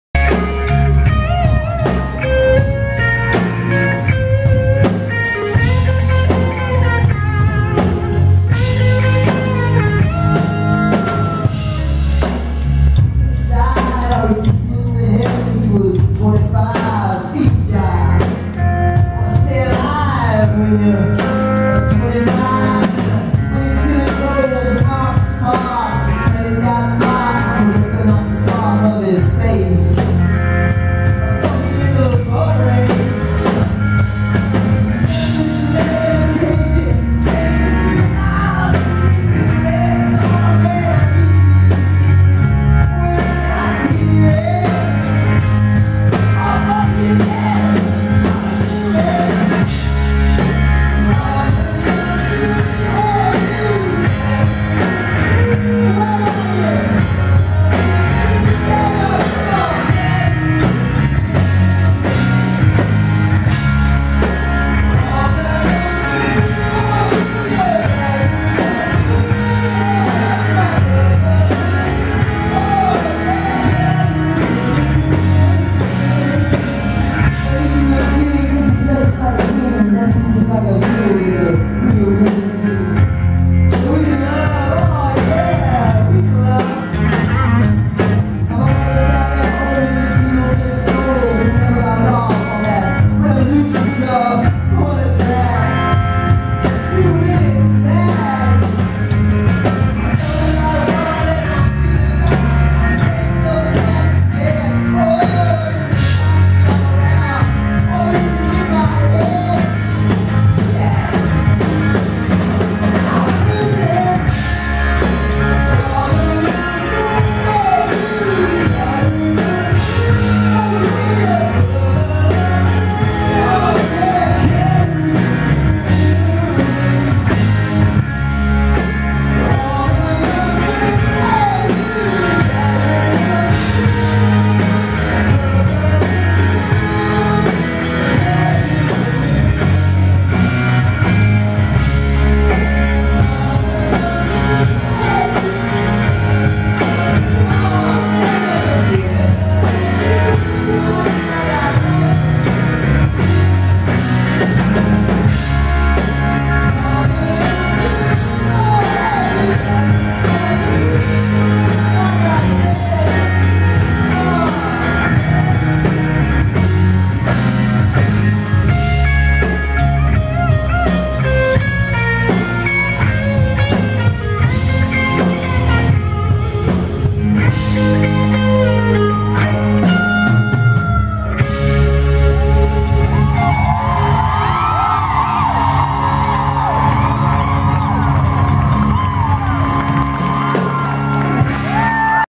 TorCon 2014 Cabaret - Mark Sheppard on drums
Mark Sheppard took to the stage to play the drums while Louden Swain (along with Richard Speight Jr. and Gil McKinney) sang.